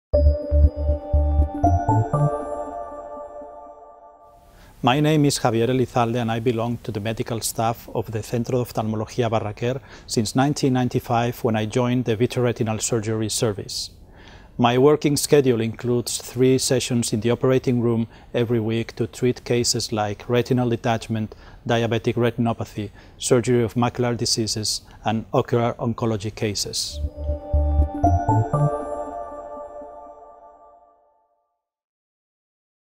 Doctor Javier Elizalde | Barraquer Ophthalmology Centre
OPHTHALMOLOGY
Bachelor in Medicine and Surgery from the Autonomous University of Barcelona (UAB) in 1990. Doctor Cum Laude in Medicine and Surgery by UAB in 2003 (National Prize Thesis). Training of specialist in ophthalmology in the MIR Barraquer Ophthalmology Centre (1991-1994). Chief of residents in 1994. Training in USA between 1995 and 1998 in pathology and surgery of the vitreous and retina at the Bascom Palmer Eye Institute (Miami, FL) with Dr. Donald Gass, in Columbia University (New York, NY) with Dr. Stanley Chang, at the Vitreous Retina Macula Consultants (New York, NY) with Dr. Lawrence Yannuzzi and in Memphis (Tennessee) with Dr. Steve Charles. Complementary Training in ocular oncology at the Wills Eye Hospital (Philadelphia, PA) with the Drs. Jerry and Carol Shields (1999).

Professional Activity
• Vitreous-Retina Surgeon (Barraquer Ophthalmology Centre)
• Deputy Coordinator of the Vitreous-Retina Department
• Coordinator of the Ocular Oncology Unit
• Vice-president of the Academic Board of the Barraquer Institute

Language
Spanish, Catalan, English, French, Italian